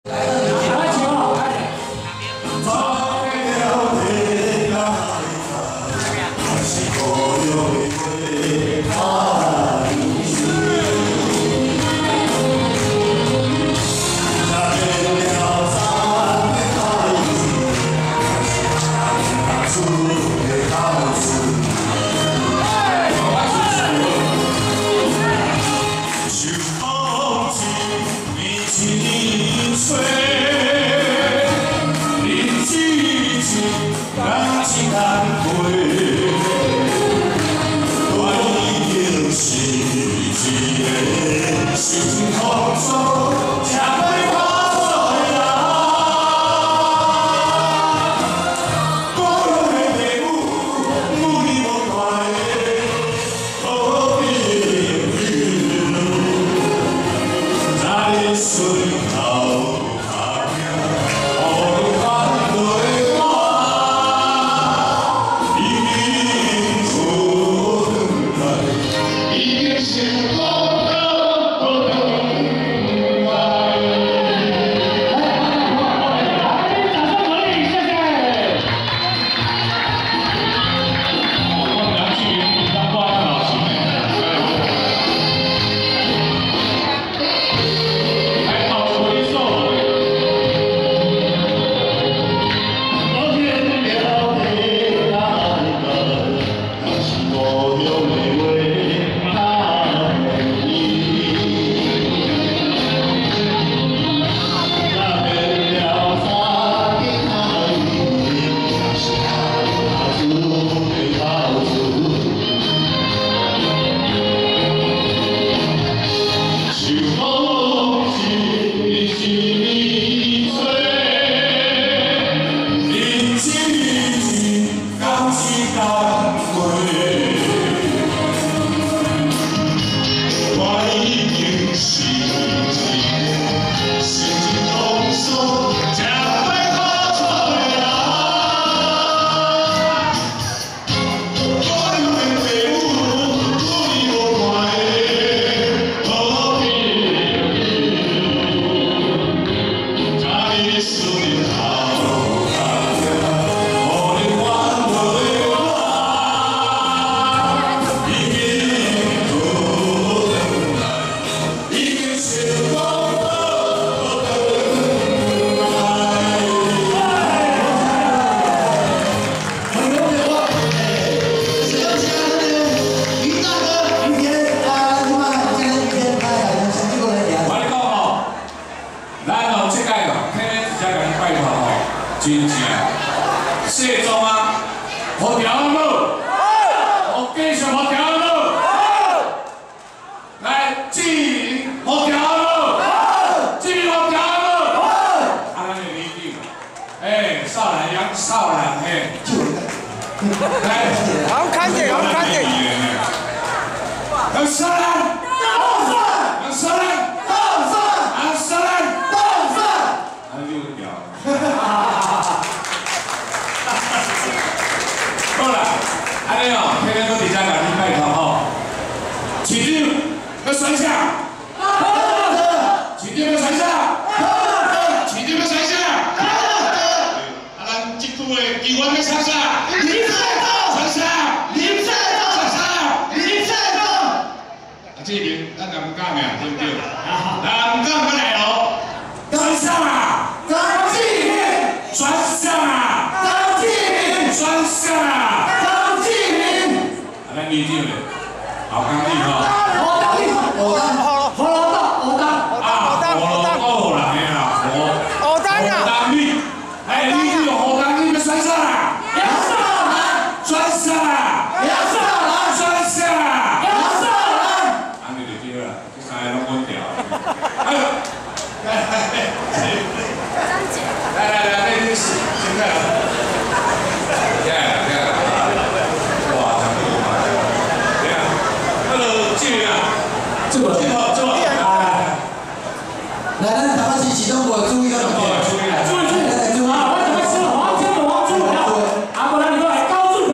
台北市葫東里 林世宗 議員 楊詔蘭 里長選舉成立歡樂會
余天 蒞臨現場祝賀